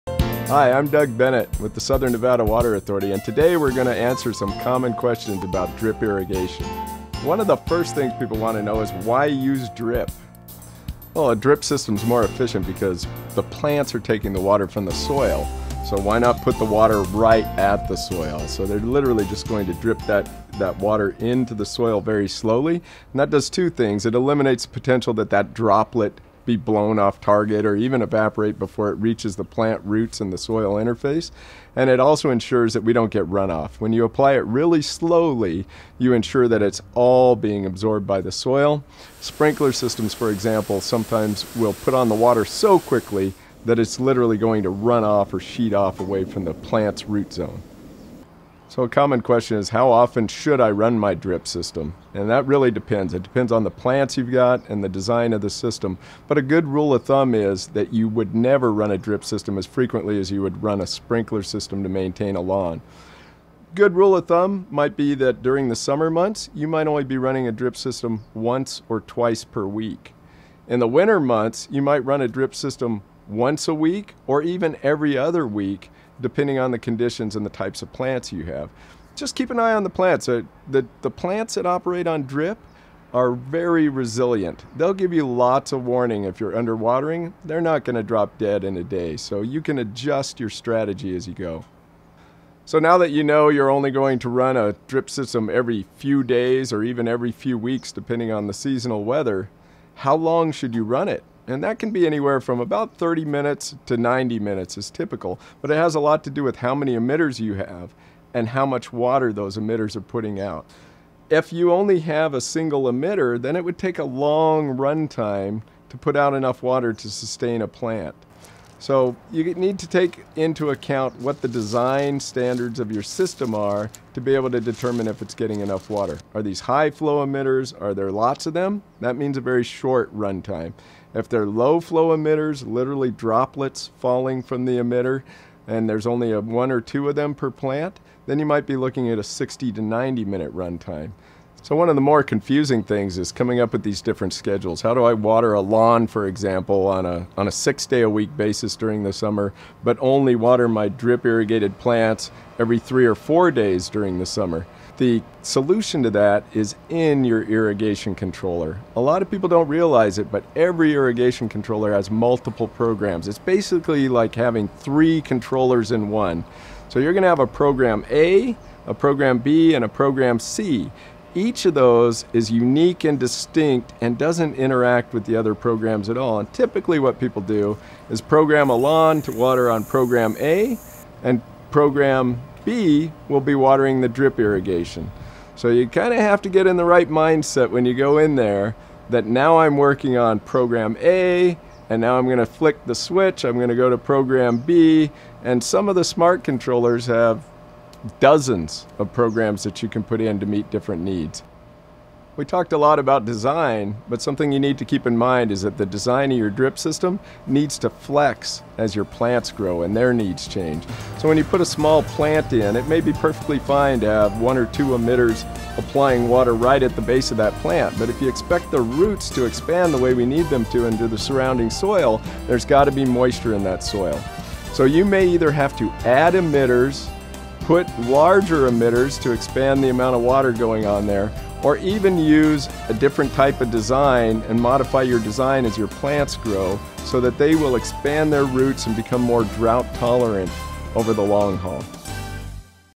Drip details
Plants prefer to sip water, not guzzle it. The best way to give your plants the right amount of water is with drip irrigation. Watch to learn how drip systems work.